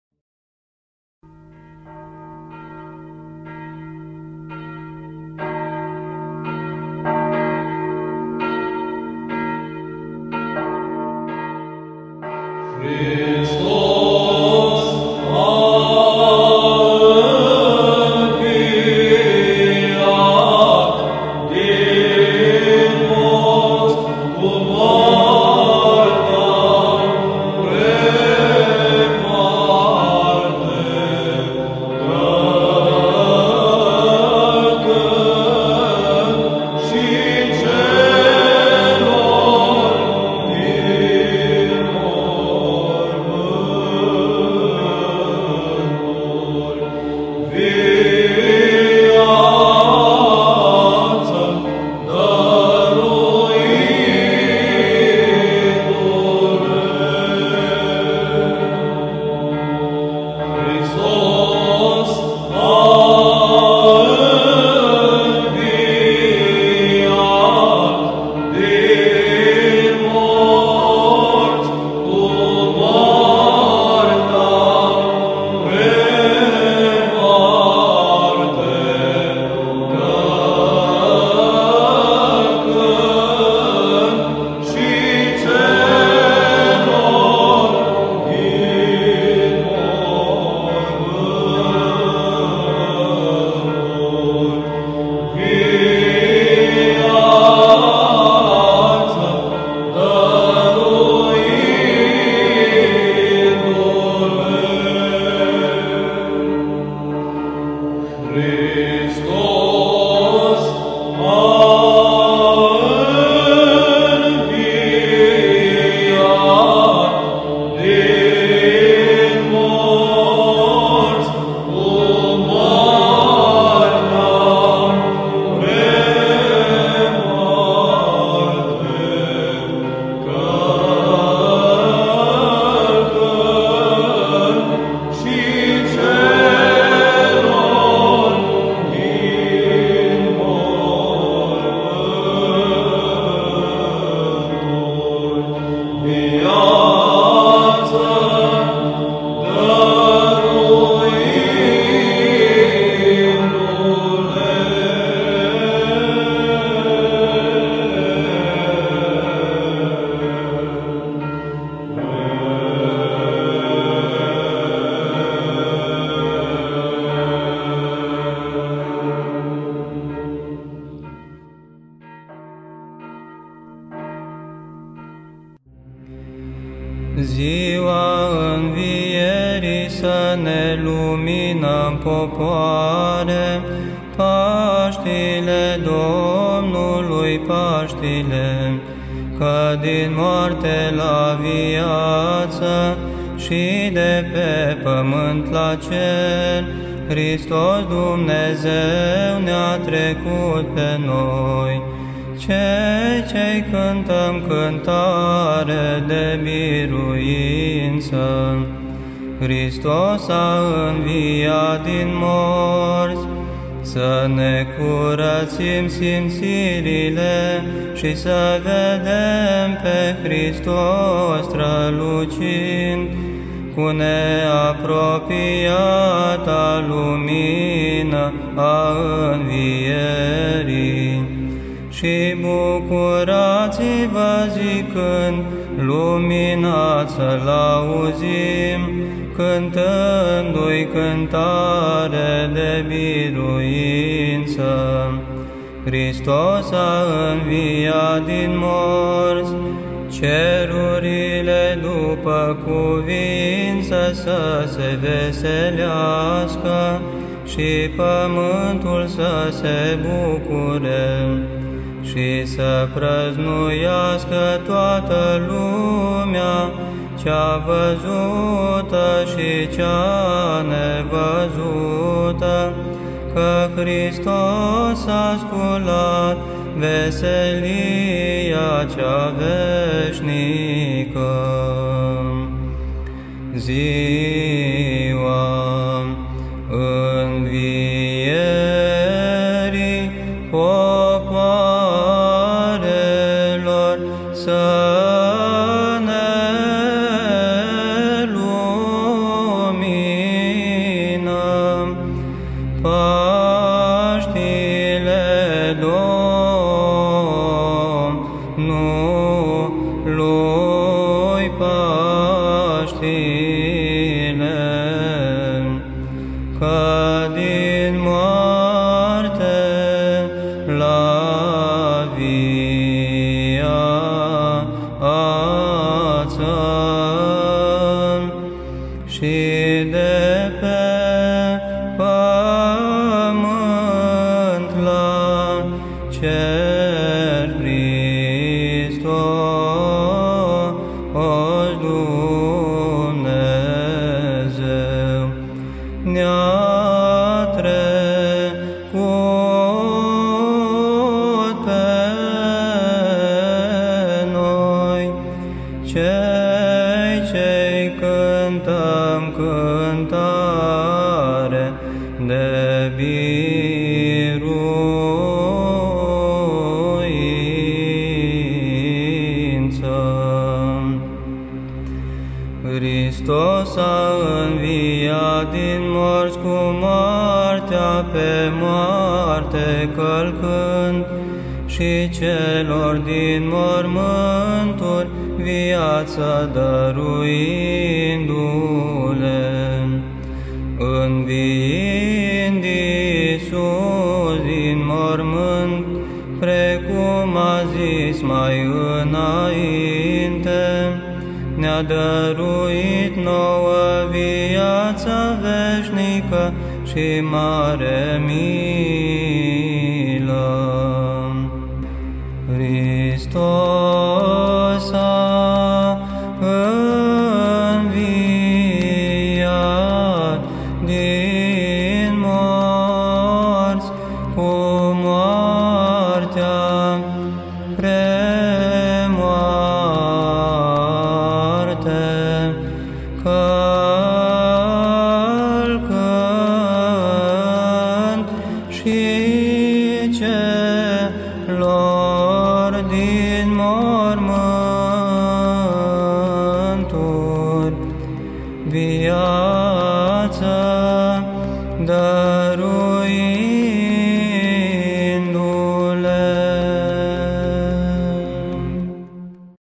O retrospectivă istorică - Parohia Ortodoxă Română Pescara
O retrospectivă istorică a Parohiei Ortodoxe Române din localitatea Pescara la înplinirea a trei ani de la punerea primei pietre de temelie de către Preasfințitul părinte Siluan Episcop al Episcopiei Ortodoxe române a Italiei.